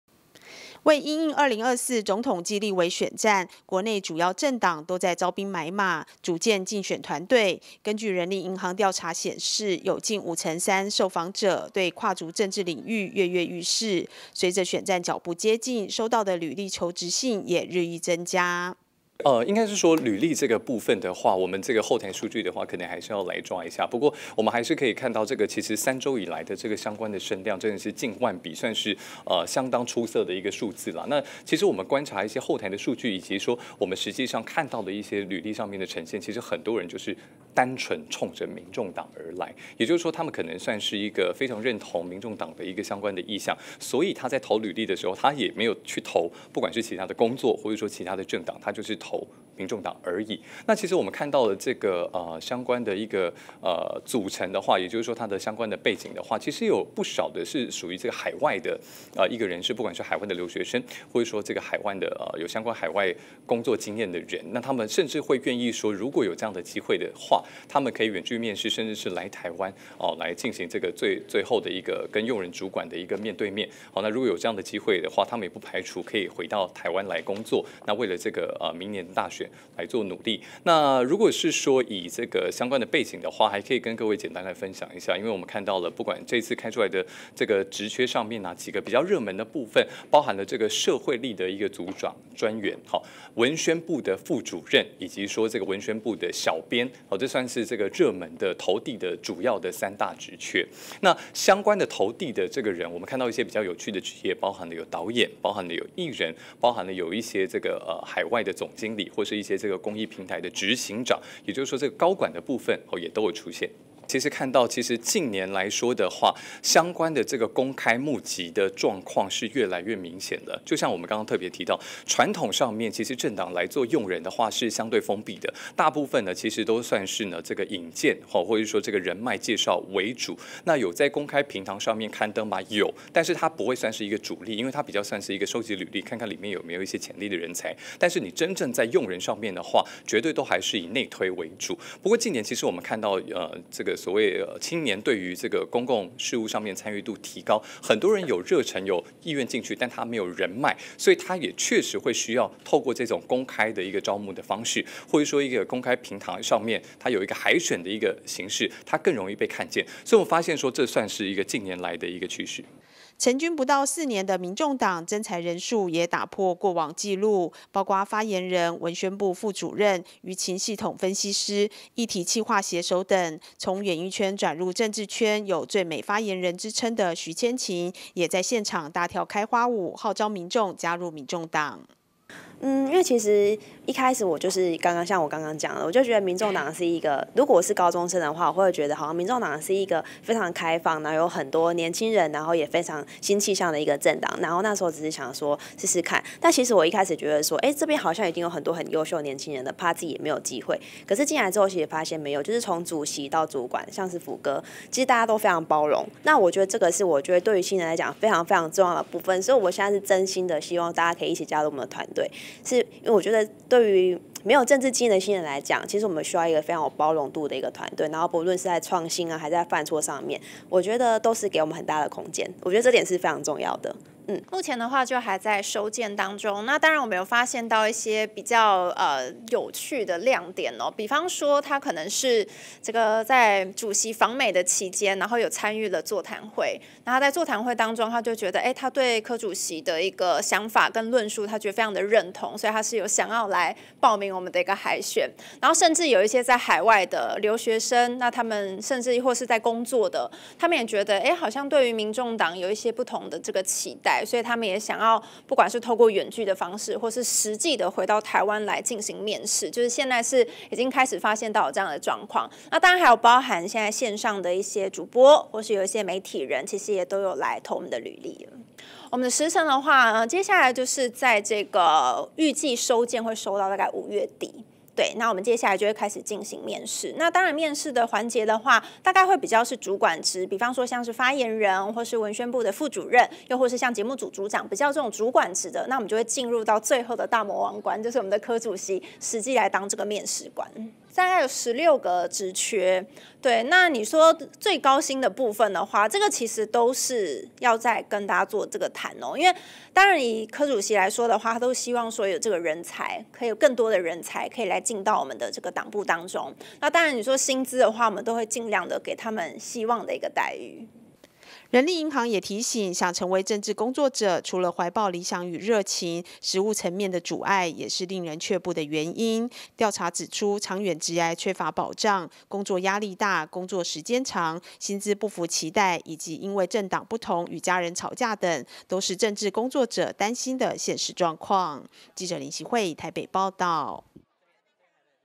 立委 選舉|求職|2024選戰鳴鼓 逾5成青年願做「造浪者」
國內首部講述政治幕僚背景的職人劇「人選之人-造浪者」未演先轟動，隨著2024總統立委選舉腳步接近，根據人力銀行針對45歲以下族群進行調查發現，有高達88%的受訪者表示，一定會去投票，其中有42.7%已有屬意候選人，更有超過五成受訪者對跨足政治領域躍躍欲試。

為因應2024總統暨立委選戰，國內主要政黨都在招兵買馬，組建競選團隊。根據人力銀行調查顯示，有近五成三受訪者，對跨足政治領域躍躍欲試，隨著選戰腳步接近，收到的履歷求職信也日益增加。

成軍不到四年的民眾黨，徵才人數也打破過往紀錄，包括發言人、文宣部副主任、輿情系統分析師、議題企劃寫手等，從演藝圈轉入政治圈，有「最美發言人」之稱的徐千晴，也大跳開花舞，號召民眾加入民眾黨。

人力銀行也提醒，想成為政治工作者，除了懷抱理想與熱情，實務層面的阻礙，也是令人卻步的原因，調查指出「長遠職涯缺乏保障」、「工作壓力大」、「工作時間太長」、「薪資不符期待」以及「因為政黨不同與家人吵架」都是政治工作者擔心的現實狀況。